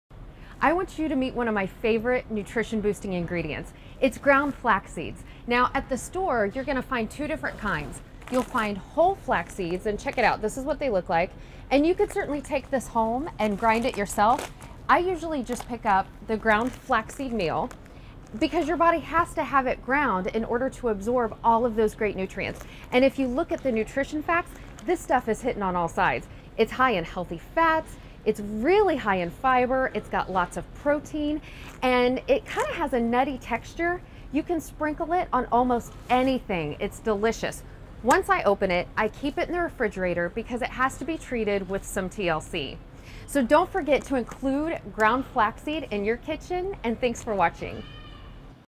Flax Seed | Your New Favorite Nutrition Booster
Listen as Annessa Chumbley, RD, introduces you to flax seed and how you can incorporate it into your daily diet!